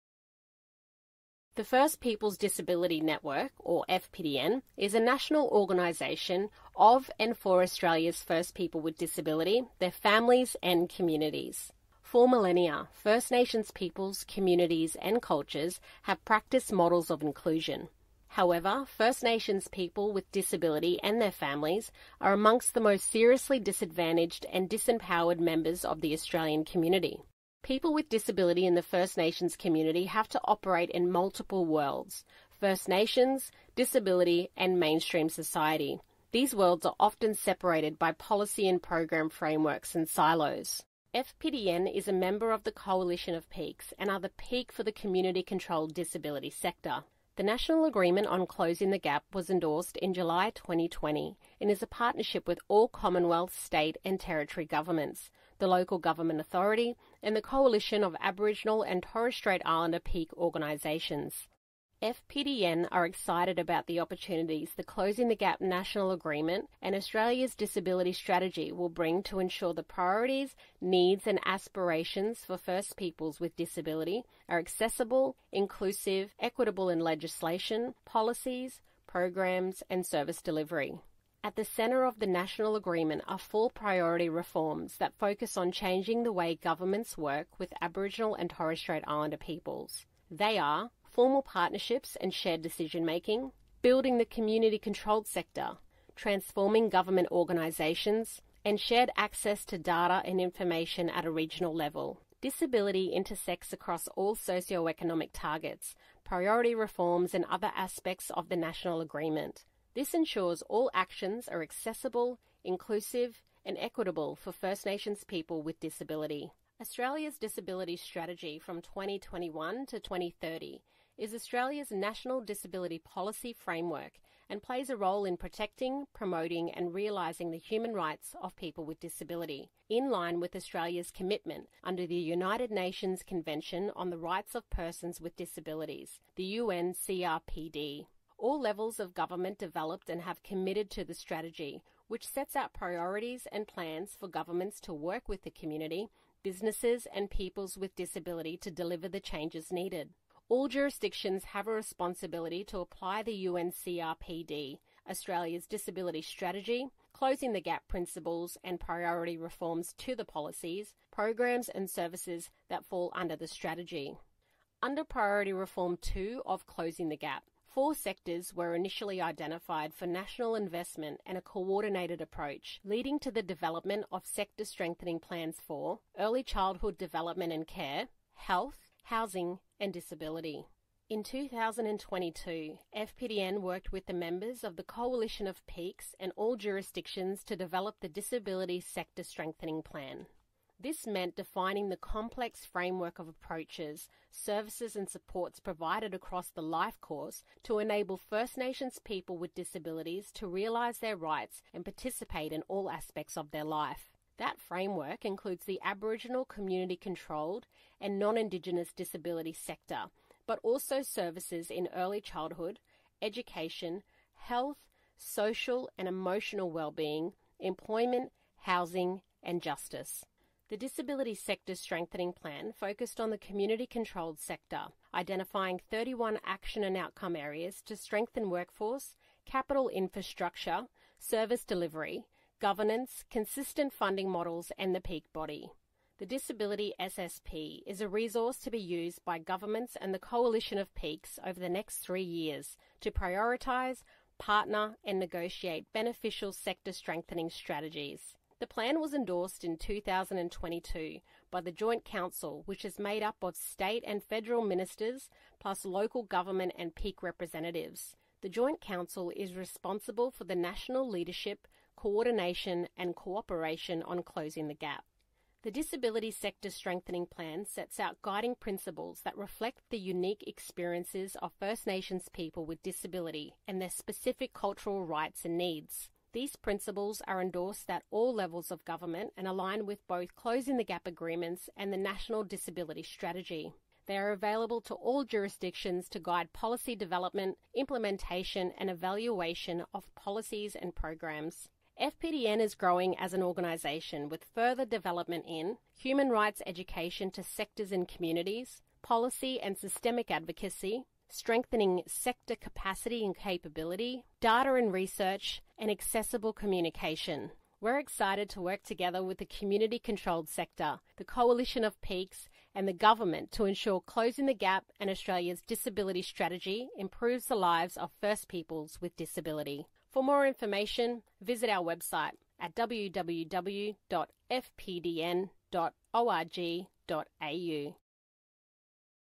FPDN, Closing the Gap and Australia’s Disability Strategy
This animation outlines the links between The National Agreement on Closing the Gap and it’s current Priority Reform Area’s, the National Disability Sector Strengthening Plan and Australia’ Disability Strategy , and the impacts on the needs, priorities and aspirations of First Peoples with Disability.

Learn how FPDN is having a positive impact on the lives of First People with disabilities